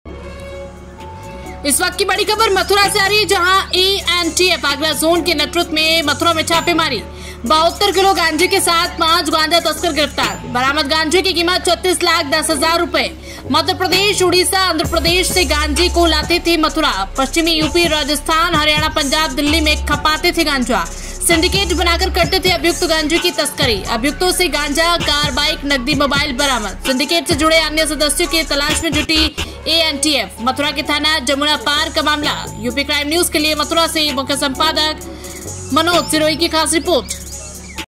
ANTF आगरा जोन के नेतृत्व में मथुरा में छापेमारी,72 किलो गांजे के साथ 5 गांजा तस्कर गिरफ्तार
UPCRIMEPRESS news
UPCRIMEPRESS News Channel